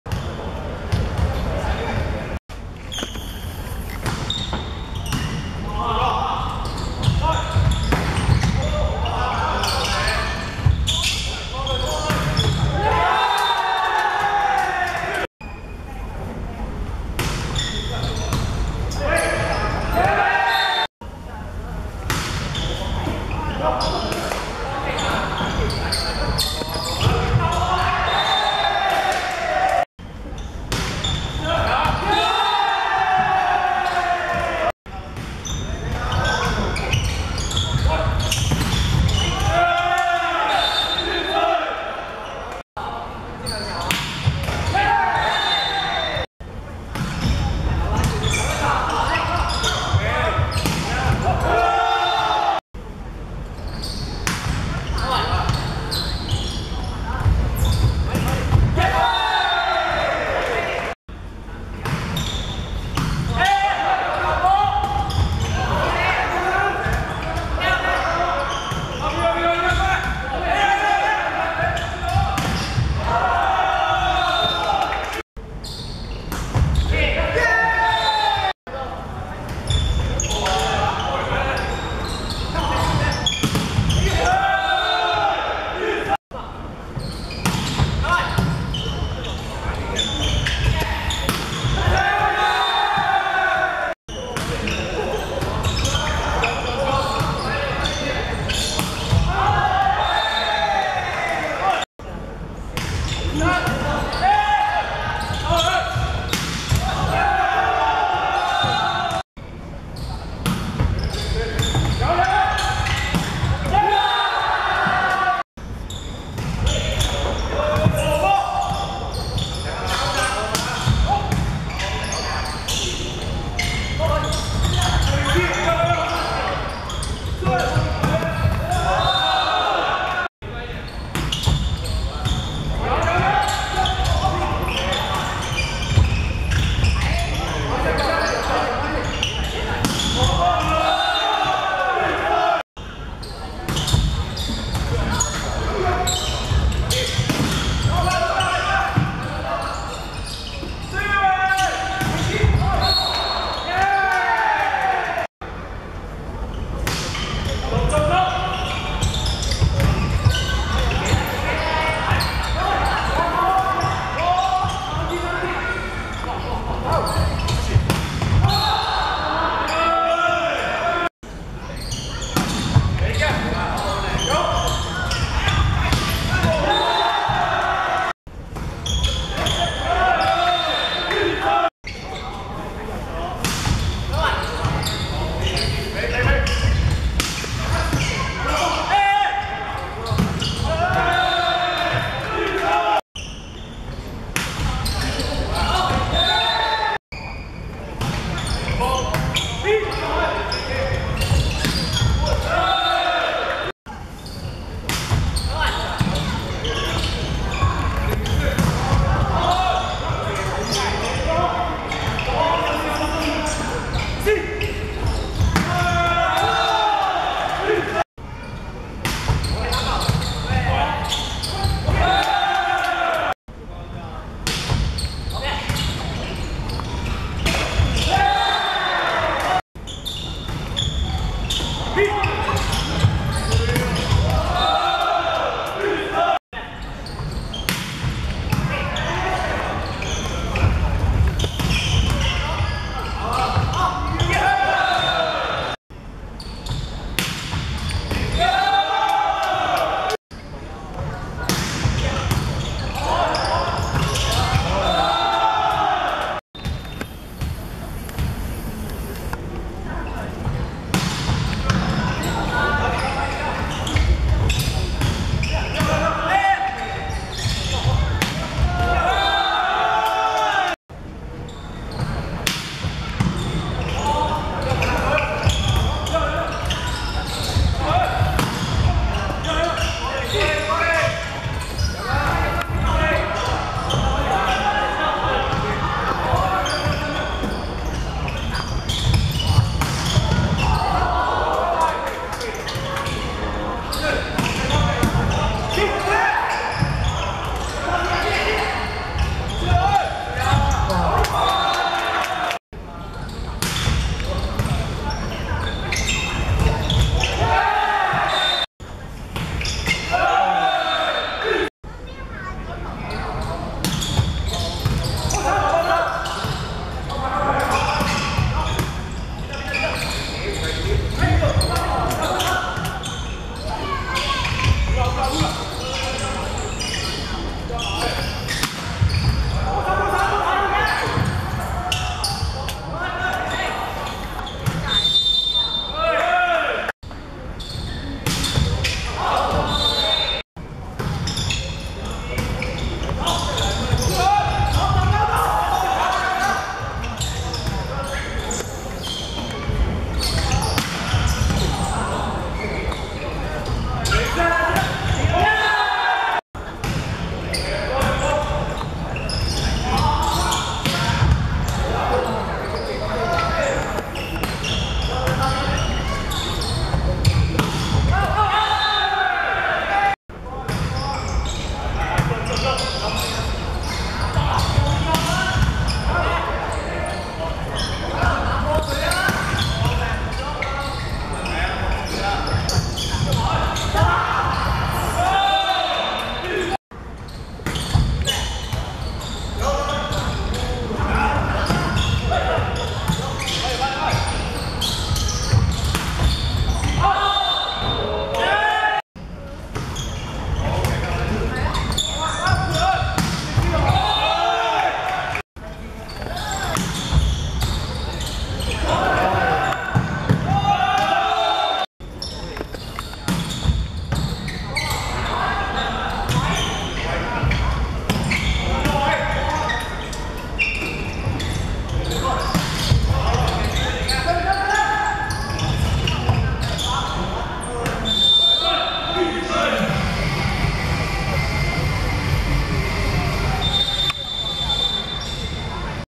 澳門vs台灣(2016.1.2.两岸四地男子排球邀請賽)第一局精華
2016.1.2.上水保榮路體育館 12:00 第一局賽果 澳門(藍) 19 : 25台灣豐原高商(黃)